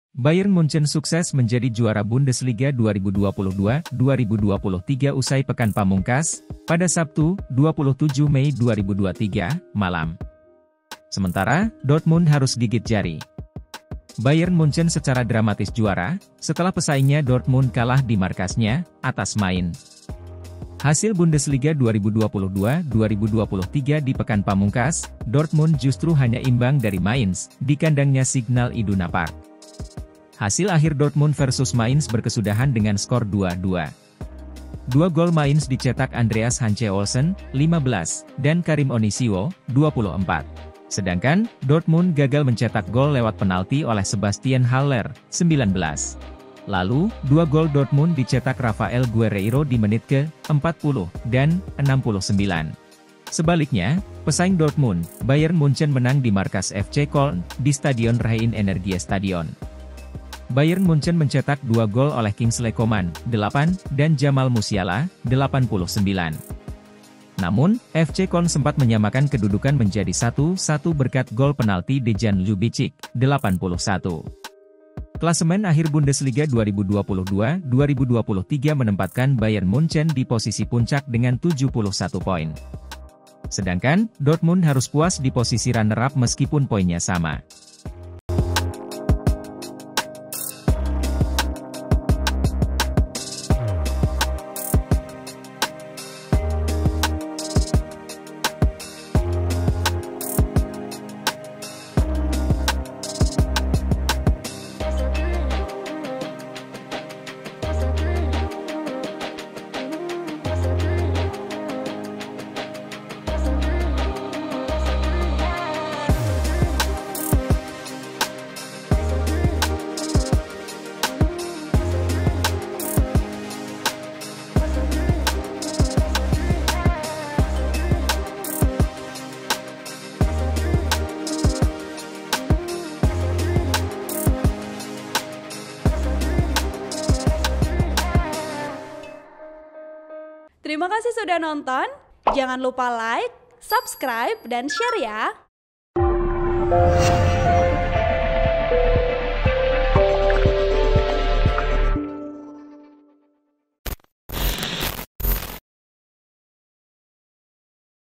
Sangat Dramatis! Bayern Munchen Juara Bundesliga! Dortmund Gigit Jari: Cek Klasemen Akhirnya
TRIBUN-VIDEO.COM - Bayern Munchen sukses menjadi juara Bundesliga 2022/2023 usai pekan pamungkas, pada Sabtu (27/5/2023) malam.

Sementara, Dortmund harus gigit jari.

Bayern Munchen secara dramatis juara, setelah pesaingnya Dortmund kalah di markasnya, atas Mainz.